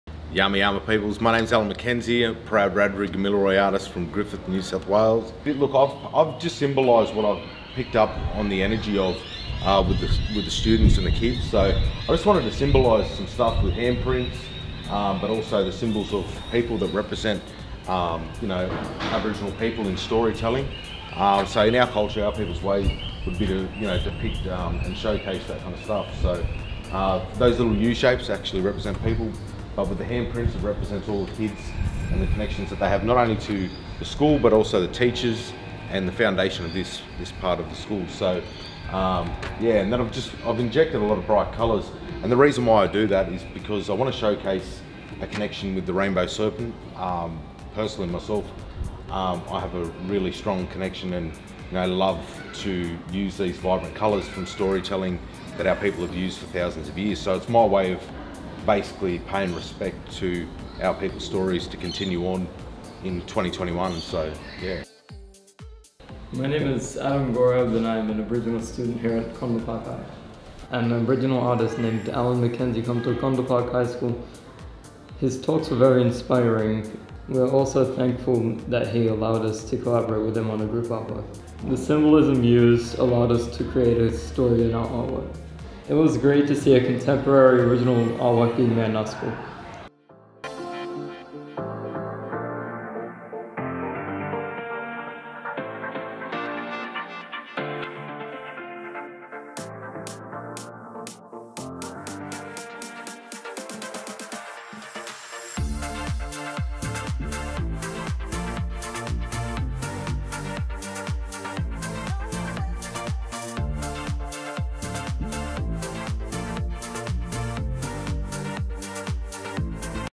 Mural Condell Park High School
Thank you Allan McKenzie for creating this beautiful Aboriginal mural for Condell Park High School.
Students had a workshop to create an original painting using traditional techniques and symbols, with a contemporary twist.